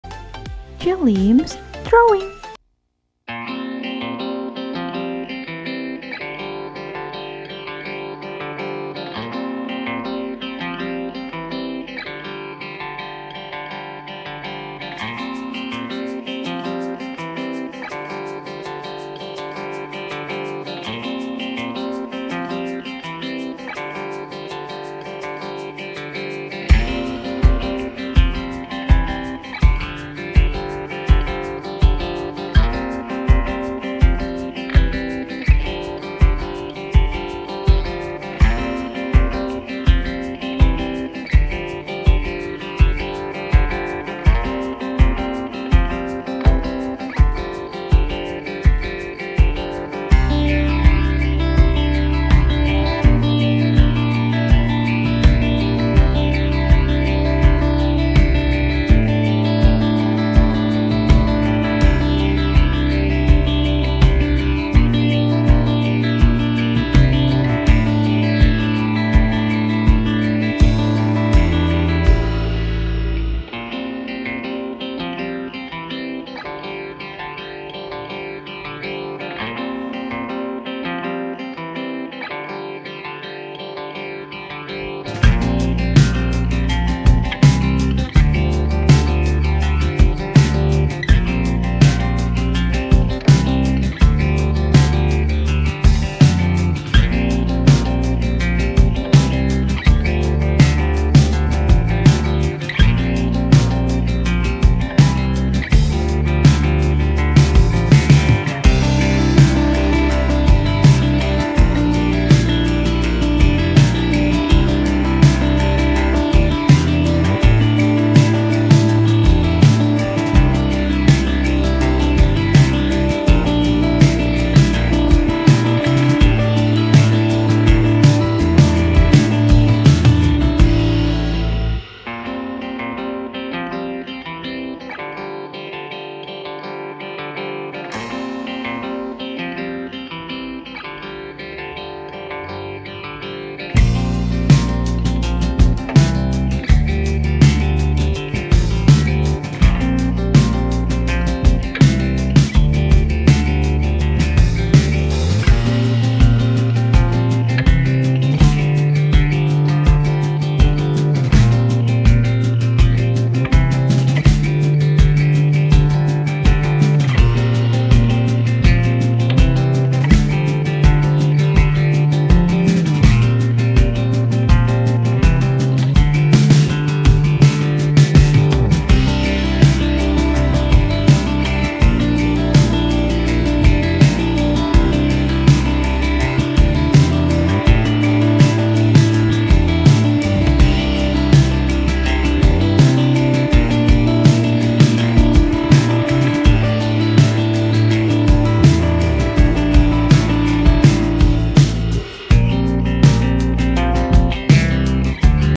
How to Draw Cute Pencil Easy Step By Step
Hello Guys, welcome to JELIMS DRAWING , in this video you will learn how to draw very cute Pencil
This Pencil is very cute and easy to draw, let's draw it together.

~Jelims~